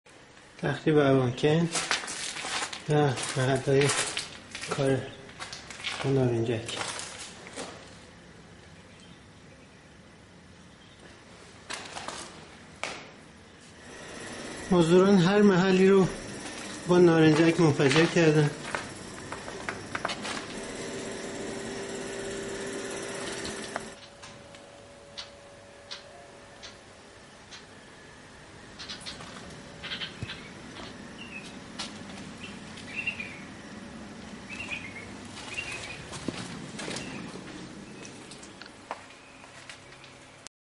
تخریب اتاقها توسط مزدوران تحت‌امر مالکی
تخریب اتاقها توسط مزدوران تحت‌امر مالکی در قتل‌عام اشرف - 10 شهریور 1392